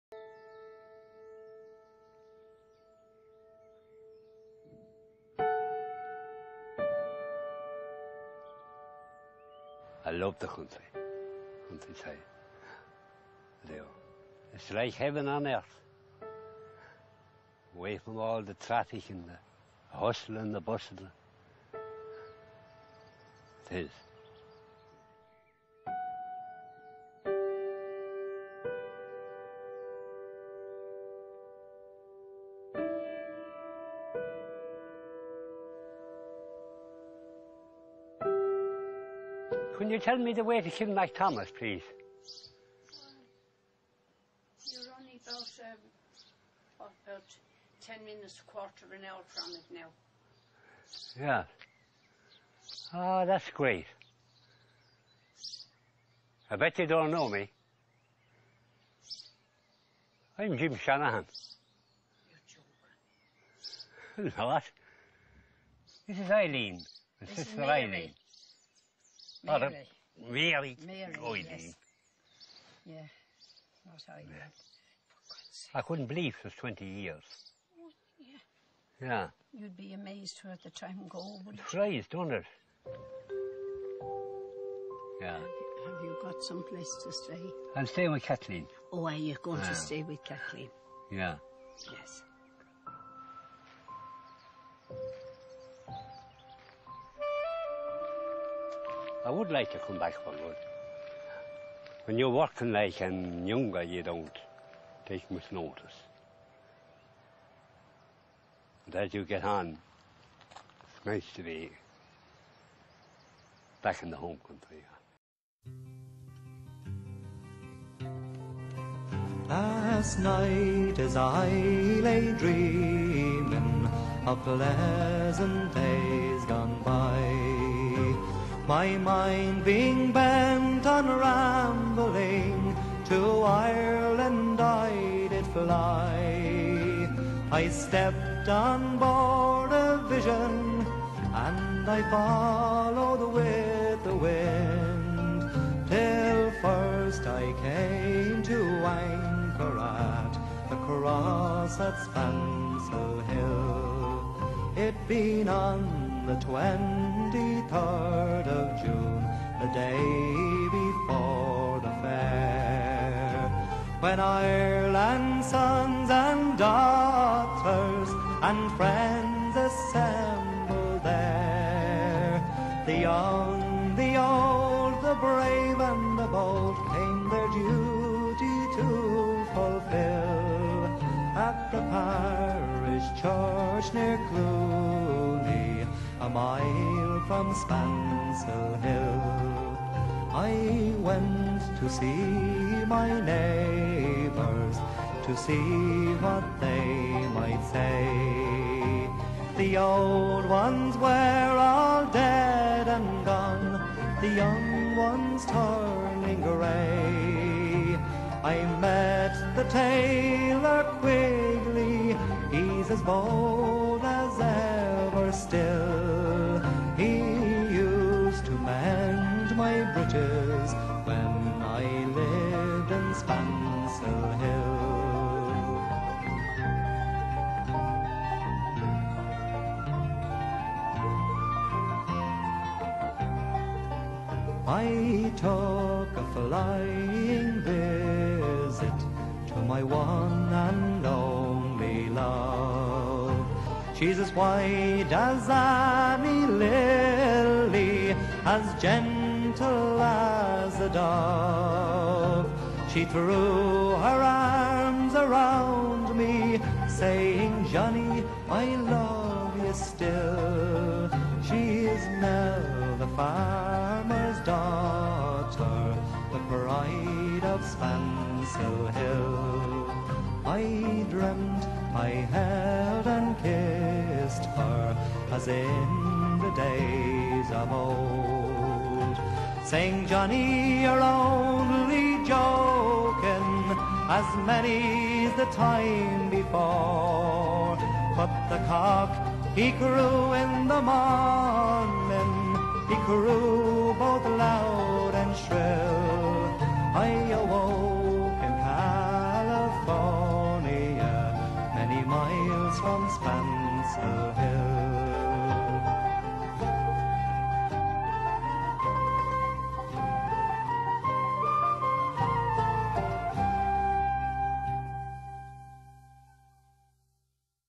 Forgotten Irish
The Intro of the video is about James Shanahan from County Waterford. Who wasn't back in Ireland for 20 years and when he went back one of his sister's don't even know him.

This video i have made here is for the Forgotten Irish, who left Ireland for a better life and went to England, America, Canada & Australia and never made it home to see Ireland. Some have been lucky to go home but not many a man or woman hasn't.

P.S. There are alot of people out there who left Ireland and dont want to make the move back home well i'll say to you move back when you think the time is right. IRELAND IS YOUR HOME.